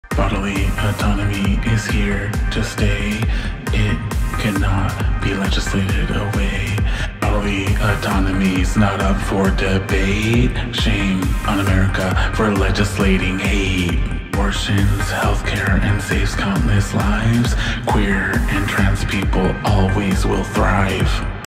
A spell for bodily autonomy
CN: This is a spell in support of abortion rights and queer, trans, and gender-variant people's rights!

Sometimes when you’re feeling all the feelings, a song bursts out from you. Sometimes when we’re meant to feel disempowered we search deep inside and find our infinite well of POWER.

Please affirm this spell by commenting any emojis or words that resonate. Please repeat and chant these words and help spread this spell by sharing it with your networks and tagging friends in the comments!